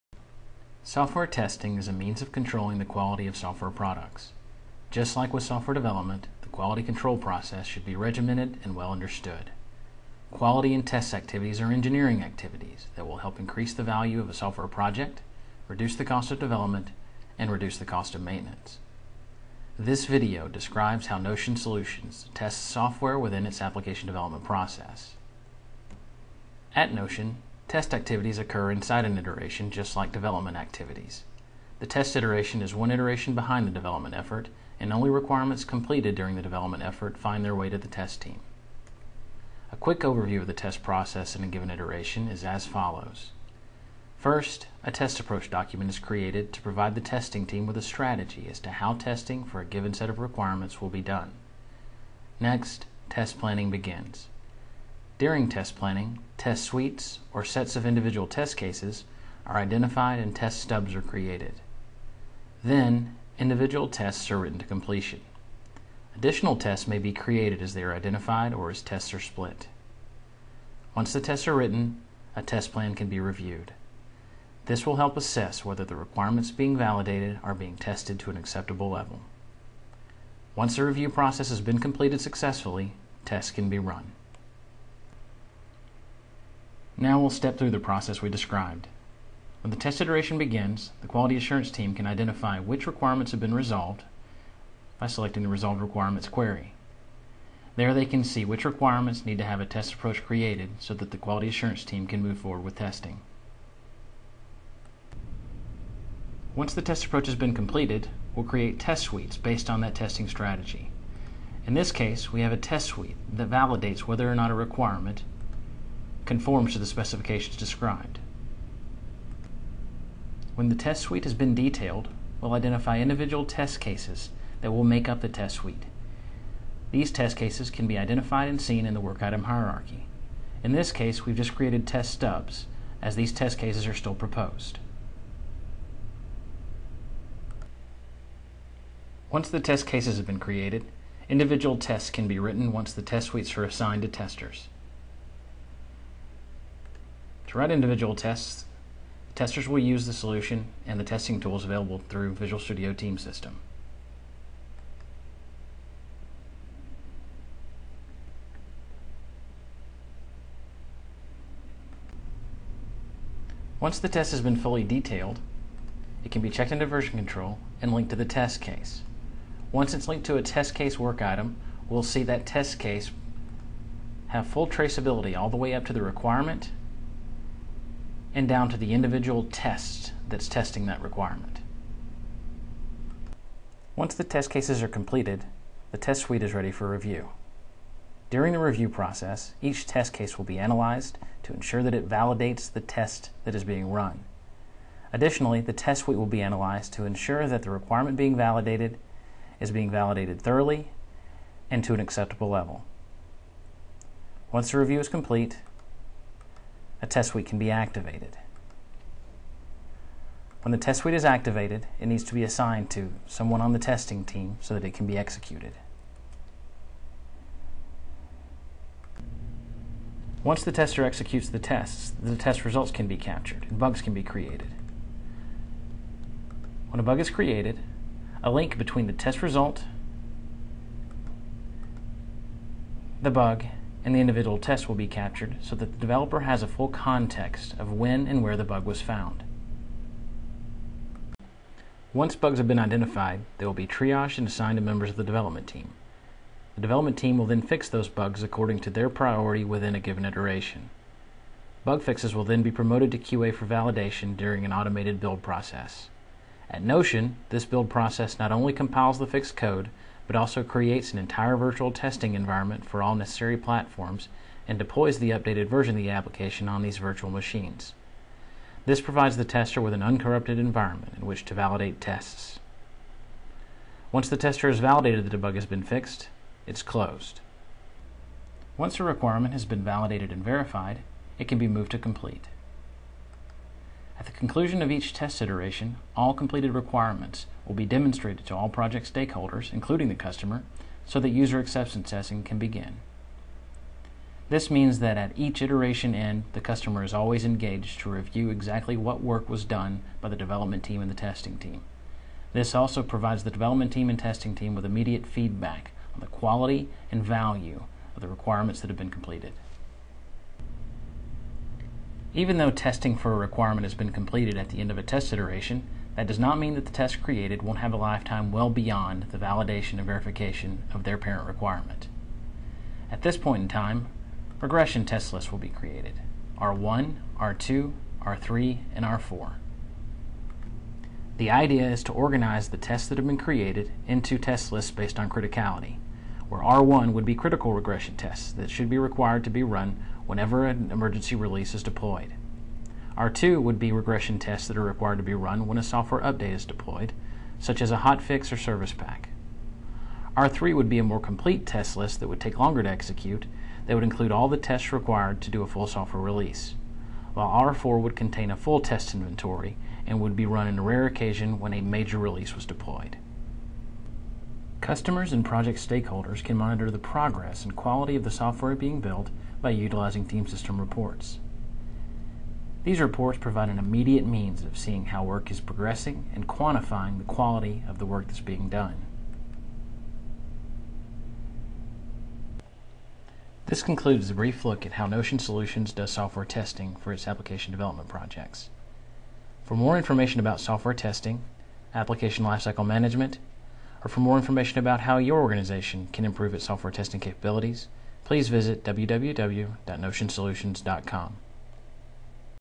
Testing Practices
This video is Notion Solutions recommendations for best Testing Practices around the use of Visual Studio Team System, both 2008 and 2010.